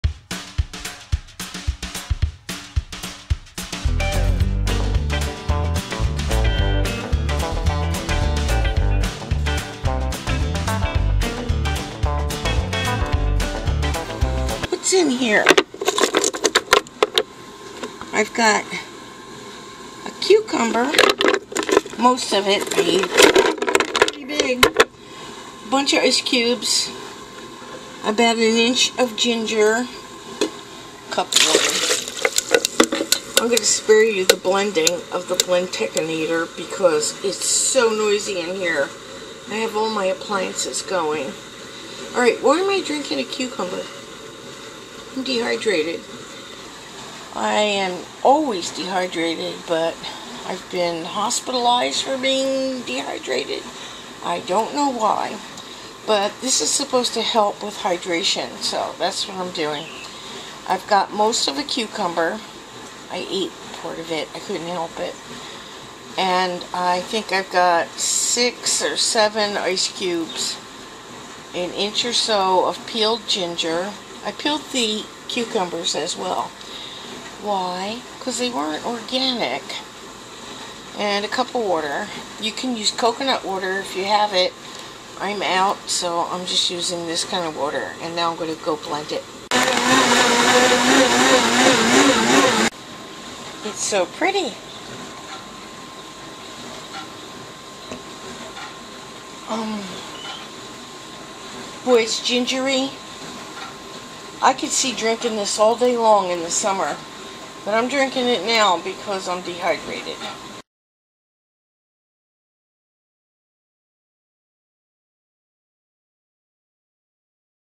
Aunt Duddie Makes A Rehydrating Cucumber Ginger Drink
I have a tendency to become dehydrated when I'm not feeling well. This is a drink I can have a few times a day without any spike in my blood glucose! Give it a try. It's good with lemon but it's great when you add ginger when you're feeling poorly.